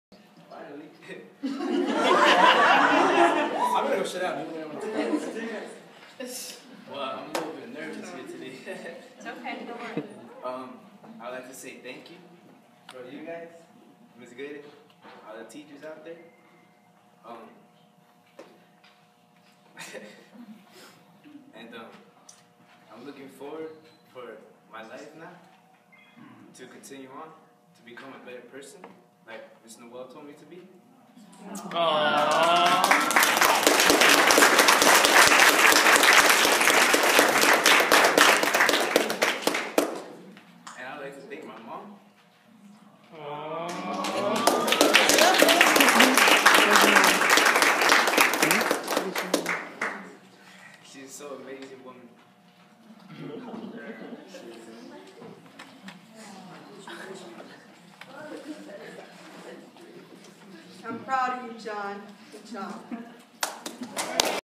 Johns surprise speech on his graduation
The RIO son graduating from HS ..moving on to the real world big and better things...becoming a better man... A mothers joy and blessing